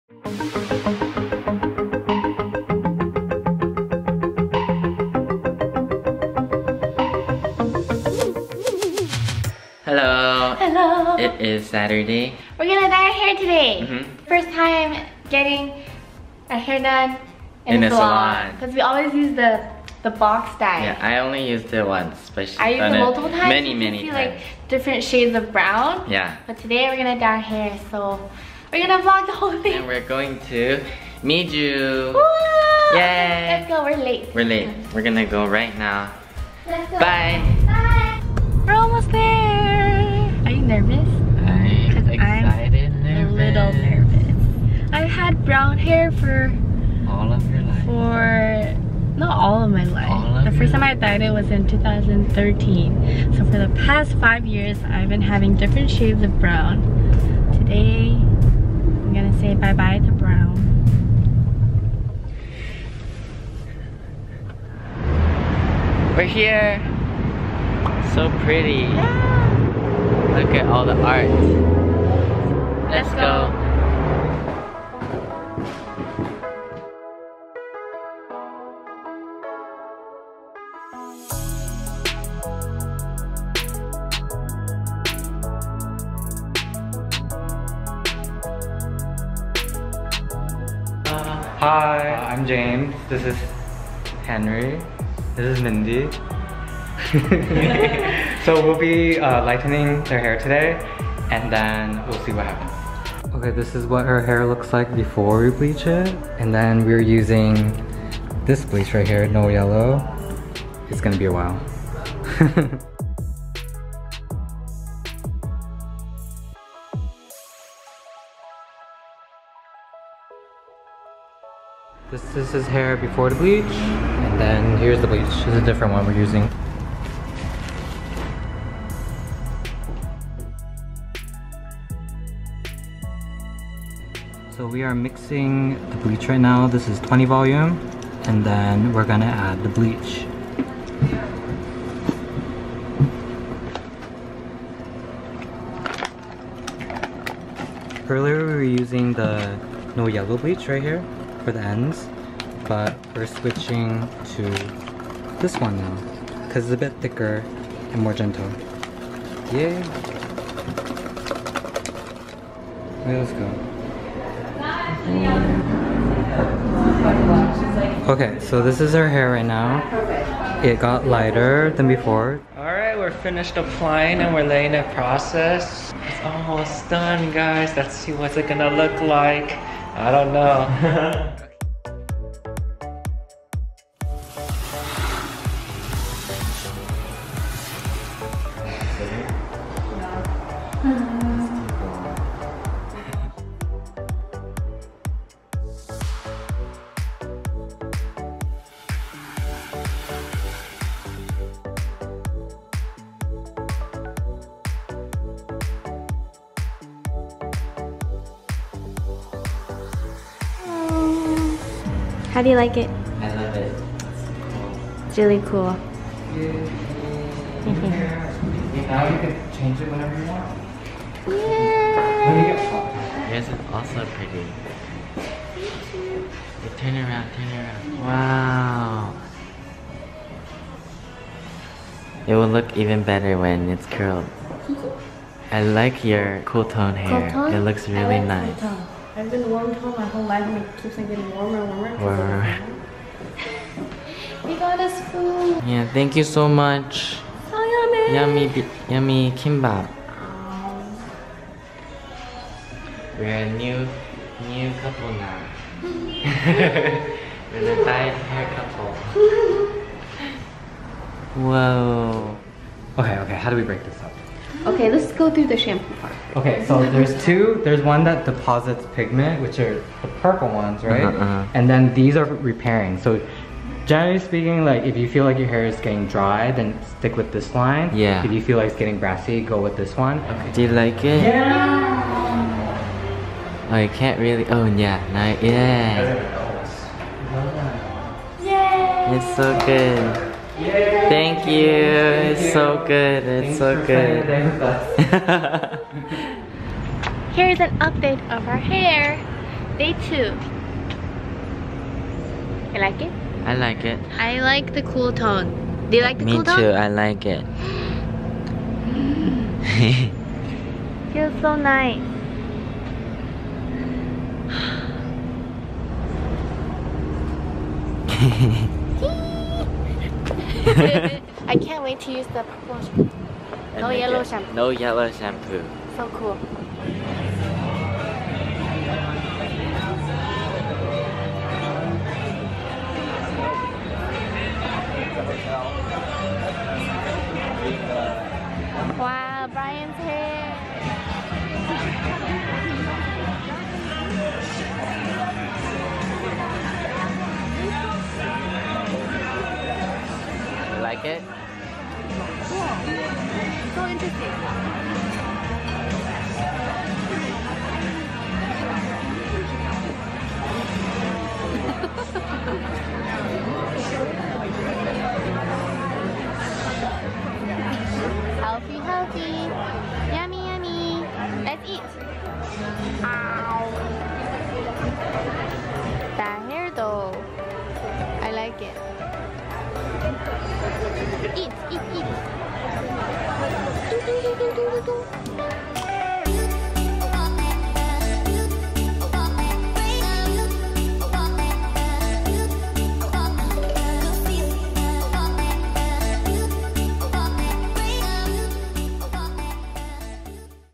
We Dyed Our Hair! | Ellen and Brian at Miju Van Salon
We dyed our hair at a salon for the first time! We both had black/brown hair before and we got it bleached and dyed by hair professionals this time~ Thank you Miju Van Salon! Watch the vlog to find out what color we dyed our hair to~ Our hair continued to fade into prettier colors few weeks after the dye so follow our social media accounts to see the full transformation :D